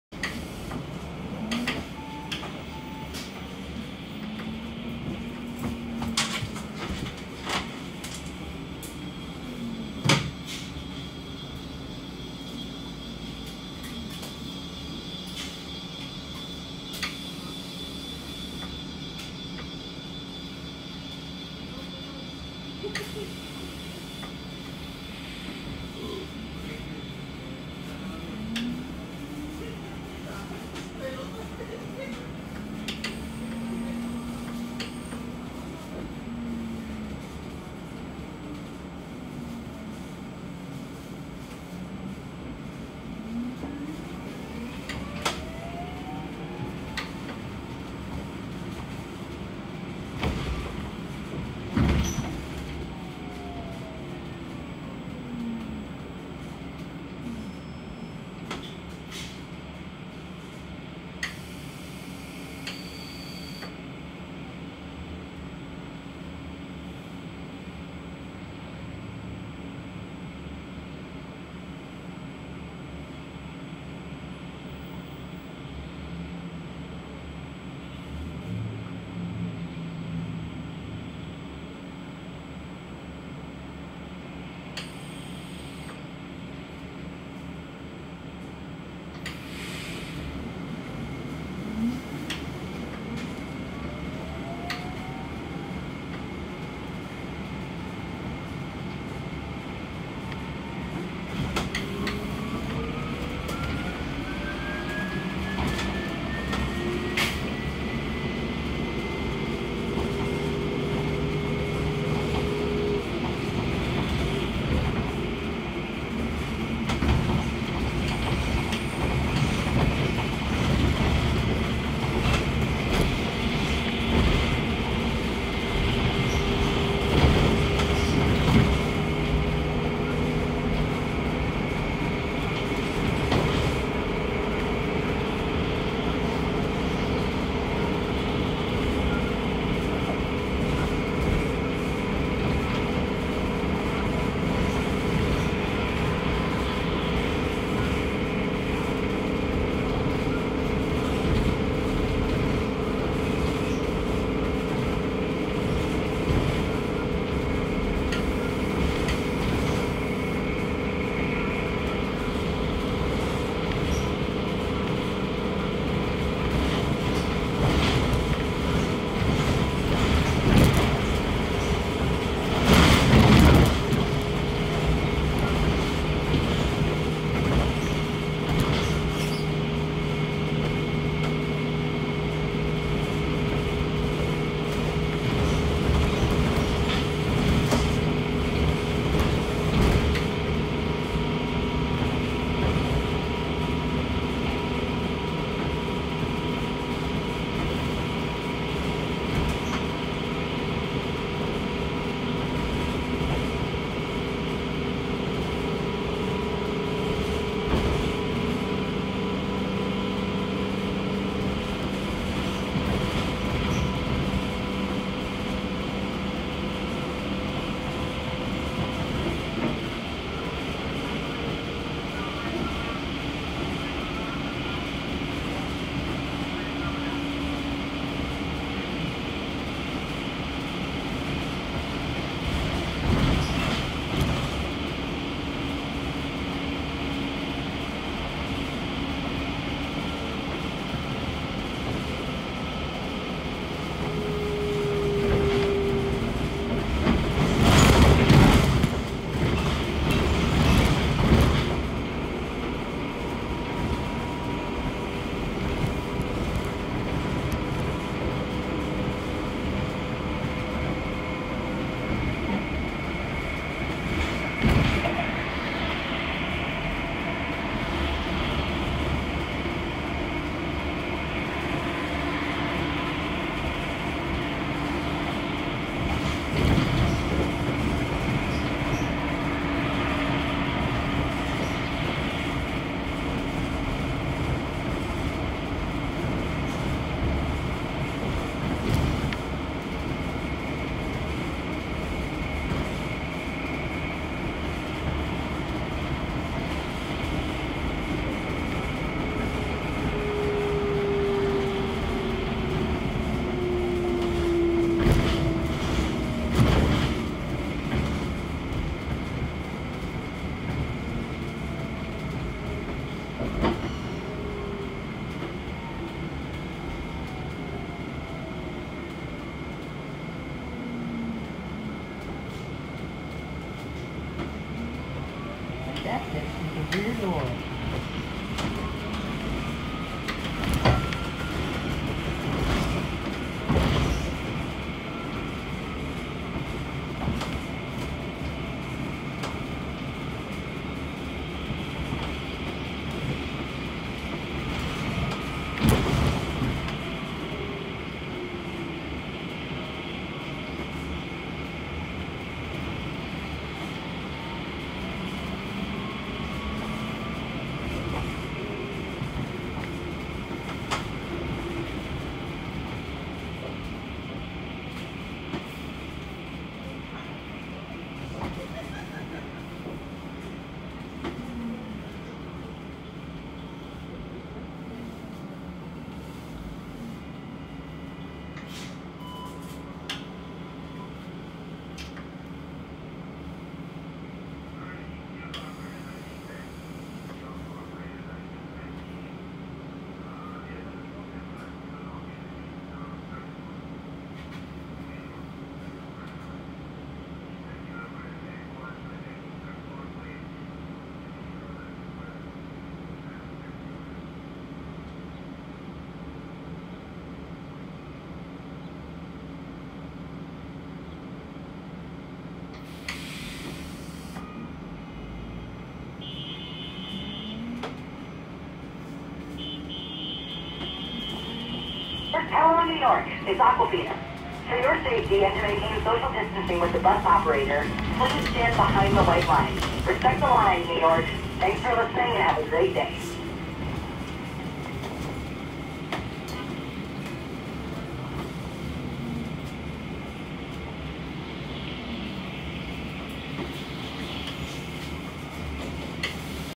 MTA: On Board Proterra BE40 Catalyst #0020 On The B39 Local Bus (Part 2/2) (03/18/2021)
This is MTA.  This is a ride onboard Proterra BE40 Catalyst #0020 on the B39 local bus from Williamsburg, Brooklyn, to the Lower East Side of Manhattan, via the Williamsburg Bridge.  This was taken on Thursday, 03/18/2021, at 5:03 PM.